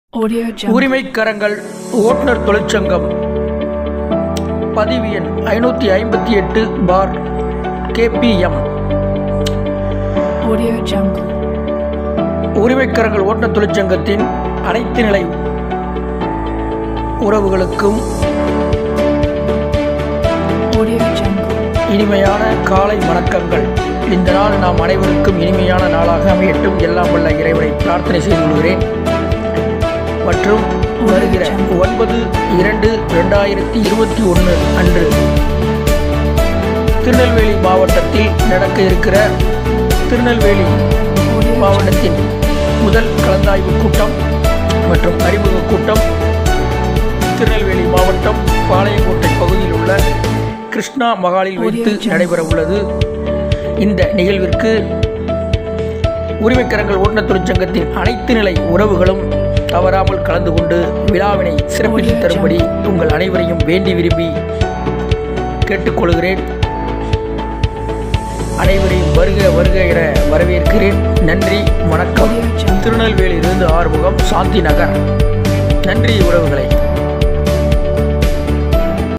உரிமை கரங்கள் முதலாம் கலந்தாய்வு
வணக்கம் 🙏
உரிமை கரங்கள் ஓட்டுநர் தொழிற்சங்கம் நடத்தும் முதலாம் கலந்தாய்வு கூட்டத்தில் வீடியோ இது
நன்றி வணக்கம் 🙏